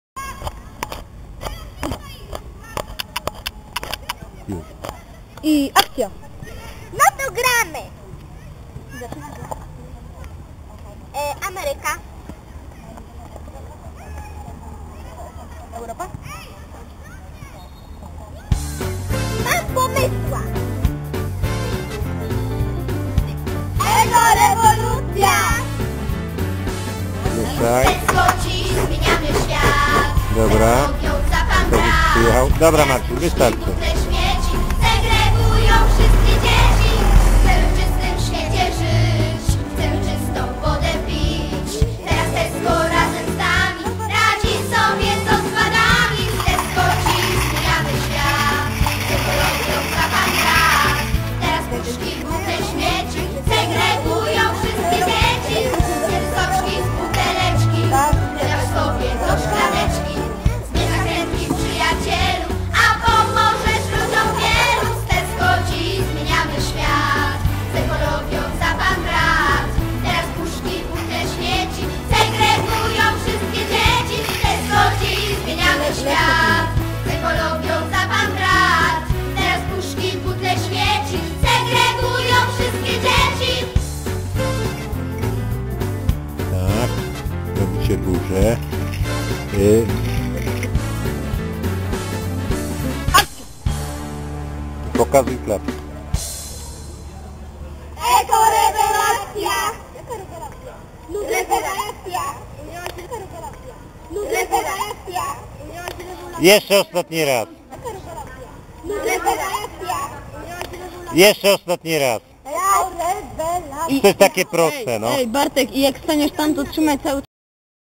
Tesco dla Szkol_Bielsko-Biala_Ekorewelacjonalisci
Film jest zlepkiem zamierzonych scen,wynikających ze scenariusza.
W miarę rozwijania się akcji następują przypadkowe i spontaniczne
zachowania aktorów. Aż w końcu nadchodzi finał, w którym pomyłka
potwierdza założenie scenariusza o niestaranności chłopców i buduje
całą scenę w sposób naturalny.

Padają stwierdzenia;"jaka rewelacja?-no rewelacja! a miałaś być
rewulacja",niezidentyfikowany głos spoza planu podpowiada
"rarewelacja"reżyser kwituje zaś słowami "to jest takie proste no".

Muzyka napisana celowo i równolegle na potrzeby całego przedsięwzięcia
jest świetnym dopełnieniem filmu,a jej autorka to bardzo skromna osoba
w przeciwieństwie do Mnie!